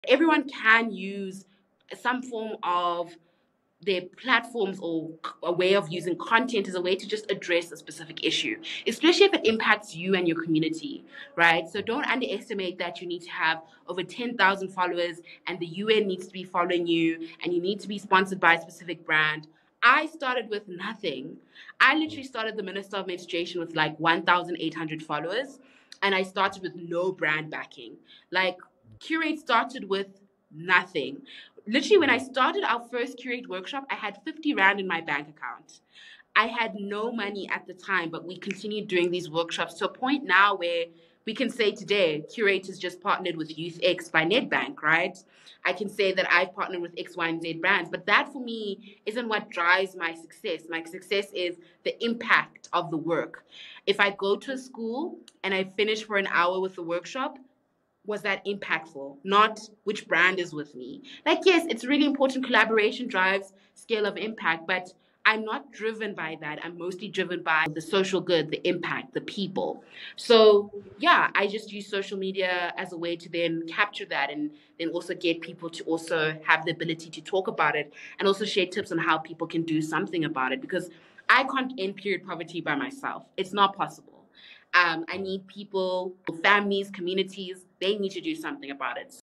Creating Impact Beyond The Number Of Followers- C.Cast With Candice Chirwa #12
In this enlightening snippet, Candice Chirwa shares a powerful message about the potential of our platforms to make a difference, regardless of our follower counts. She started her journey with just 1,800 followers and no brand sponsorship, proving that impact transcends numbers. Qrate, too, began from the ground up, growing through dedicated workshops and meaningful work.

For Candice, success is driven by the desire to create social good and connect with people. She emphasizes the importance of using social media to capture stories, foster conversations, and share solutions. While collaboration is crucial for scaling impact, her main driving force is making a meaningful difference.

Join us in this conversation as we explore how we can leverage our platforms to create positive change, no matter the size of our audiences. It's a reminder that together, we can address critical issues and empower our communities